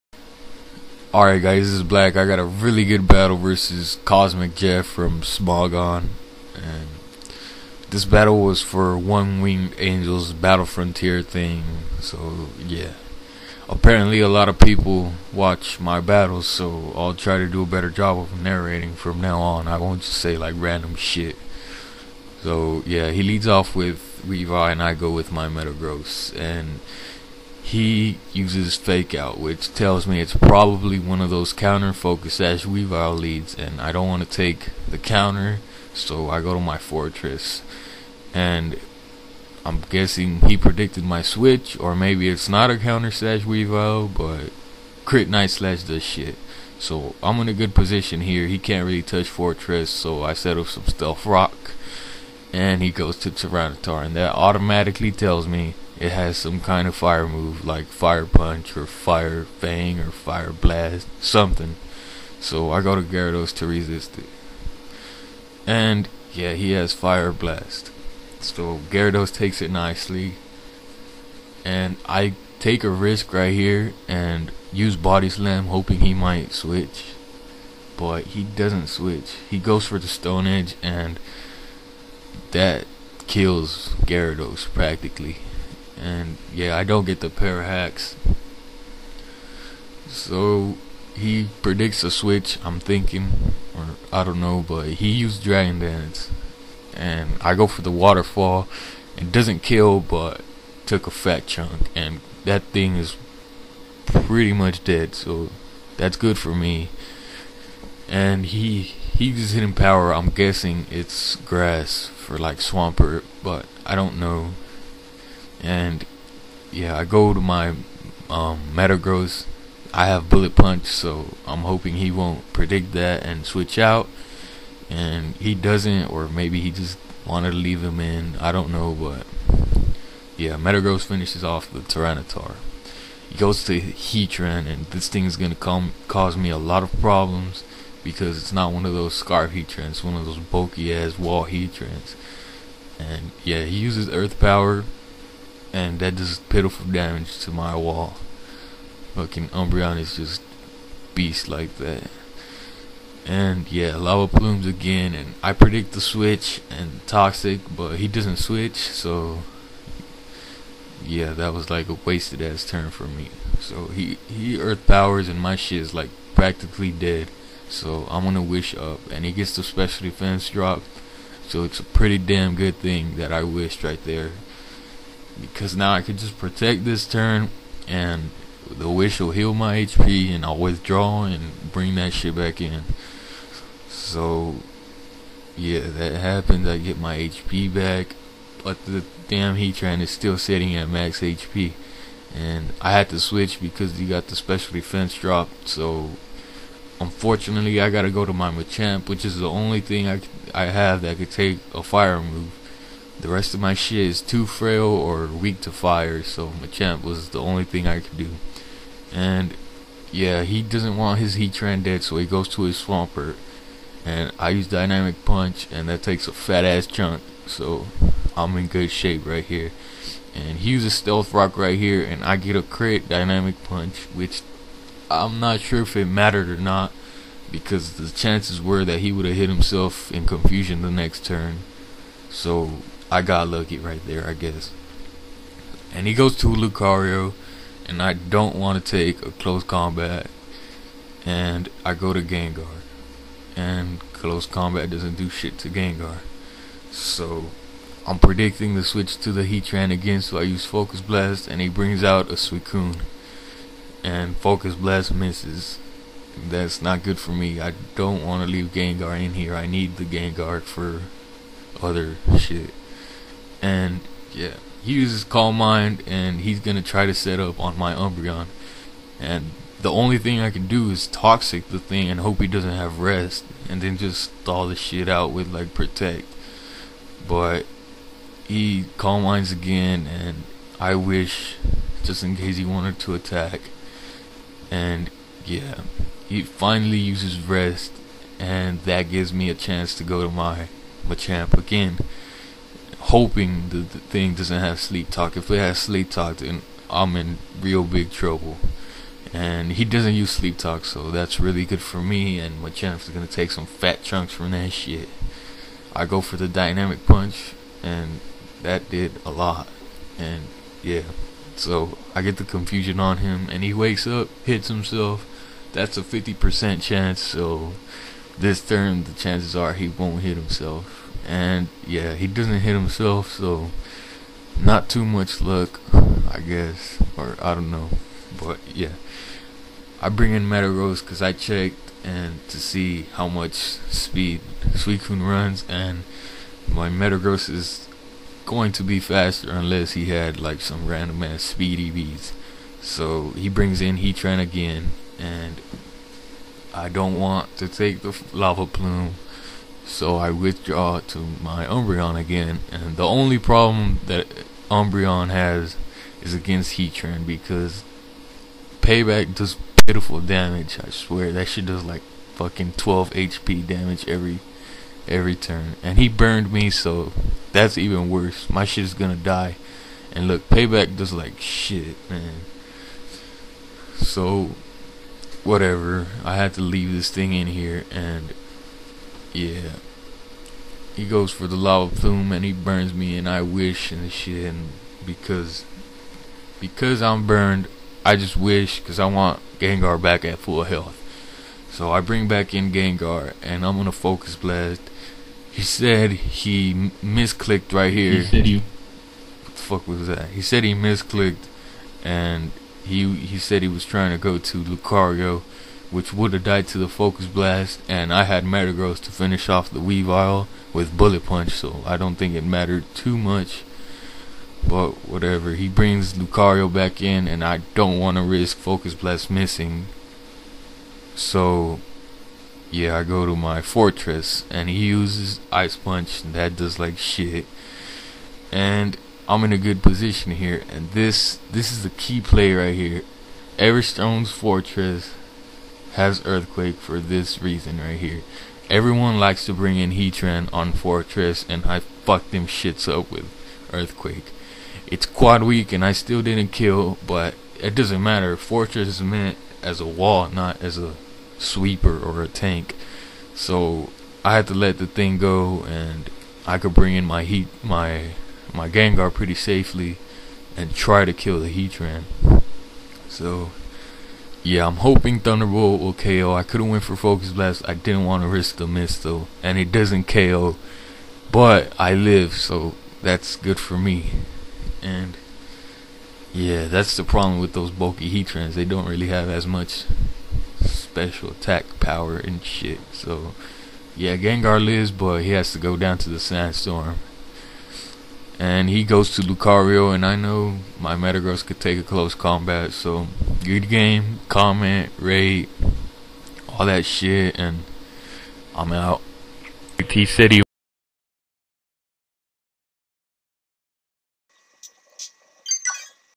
BlakBlastoise vs CosmicJeff
This was a battle of epic proportions. Jeff is a leader in OWAs Battle frontier thing. When the battle was taking place I didn't even know He was a leader, I thought he just wanted a battle. If I had known, I would have taken a different team and probably wouln't have recorded the battle.